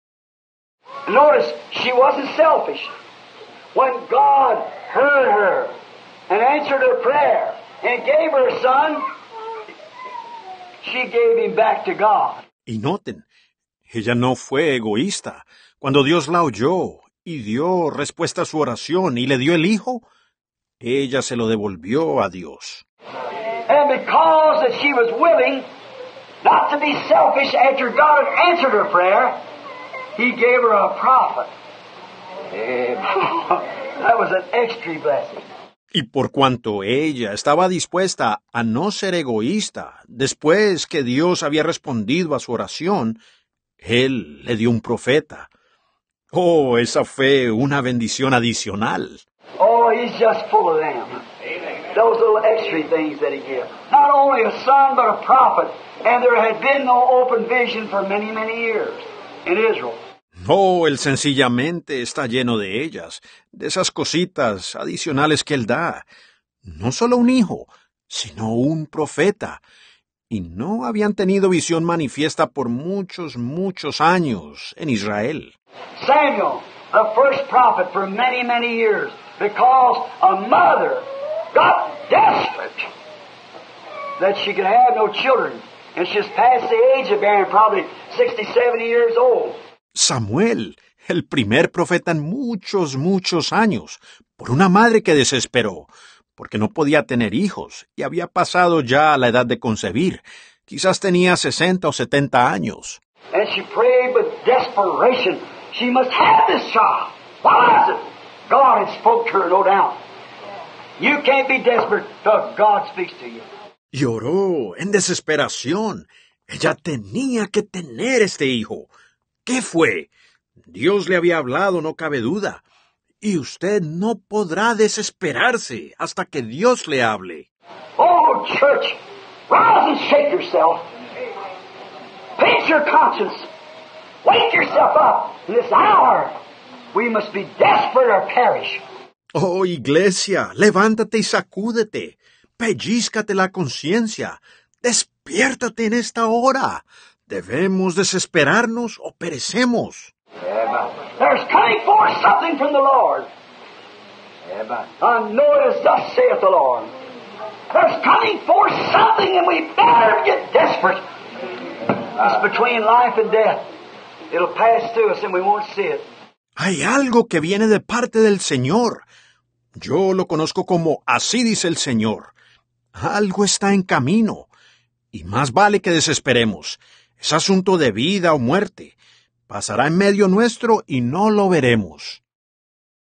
"Desperaciones" Rv. William Branham
63-0901E

Desperaciones

Y noten, ella no fue egoísta. Cuando Dios la oyó, y dio respuesta a su oración y le dio el hijo, ella se lo devolvió a Dios. Y por cuanto ella estaba dispuesta a no ser egoísta después que Dios había respondido a su oración, Él le dio un profeta. ¡Oh, esa fue una bendición adicional! ¡Oh, Él sencillamente está lleno de ellas, de esas cositas adicionales que Él da! No sólo un hijo, sino un profeta. Y no habían tenido visión manifiesta por muchos, muchos años en Israel. Samuel, el primer profeta en muchos, muchos años, por una madre que desesperó; porque no podía tener hijos y había pasado ya la edad de concebir, quizás tenía sesenta o setenta años. Y oró en desesperación, ¡ella tenía que tener este hijo! ¿Qué fue? Dios le había hablado, no cabe duda.
Y Ud. no podrá desesperarse hasta que Dios le hable. ¡Oh, Iglesia, levántate y sacúdete! ¡Pellízcate la conciencia, despiértate en esta hora! ¡Debemos desesperarnos o perecemos! ¡Hay algo que viene de parte del Señor! Yo lo conozco como ASÍ DICE EL SEÑOR. Algo está en camino y más vale que desesperemos. Es asunto de vida o muerte. Pasará en medio nuestro y no lo veremos.

📖 Pan diario

1 Pedro 4:17

Porque es tiempo de que el juicio comience por la casa de Dios; y si primero comienza por nosotros, ¿cuál será el fin de aquellos que no obedecen al evangelio de Dios?